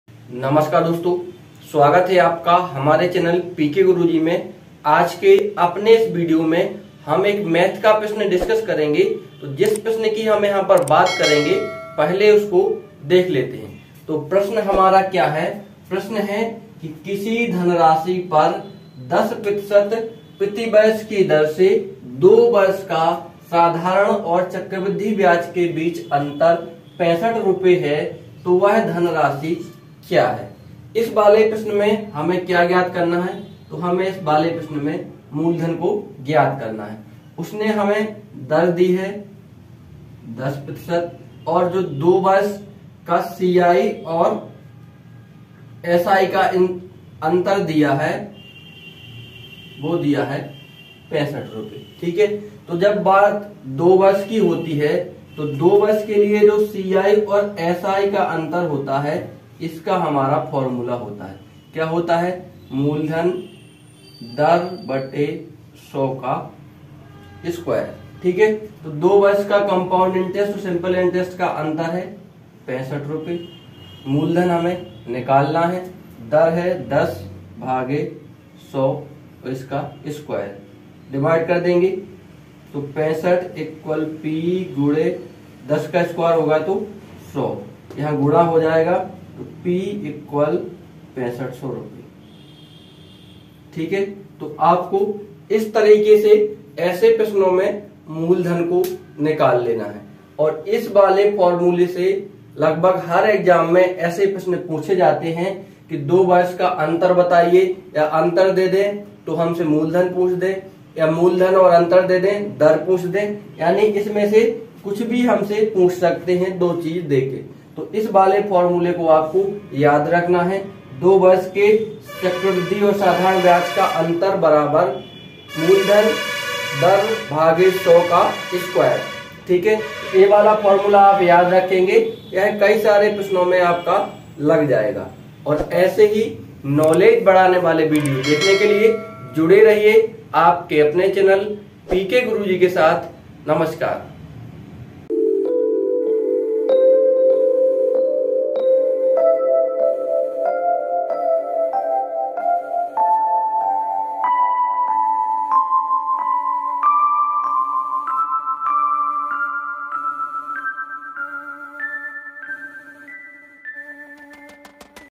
Compound interest । चक्रवृद्धि ब्याज । C.I. Trick। Maths । Mathematics । Maths with PKGURUJee
किसी धन राशि पर 10% प्रतिवर्ष की दर से 2 वर्ष का साधारण और चक्रवृद्धि ब्याज के बीच का अंतर 65 है वह धन राशि क्या है

The difference between simple and compound interest on a sum of money at 10% per annum for 2 years is 65 what is that sum

kisee dhan raashi par 10% prativarsh kee dar se 2 varsh ka saadhaaran aur chakravrddhi byaaj ke beech ka antar 65 hai vah dhan raashi kya hai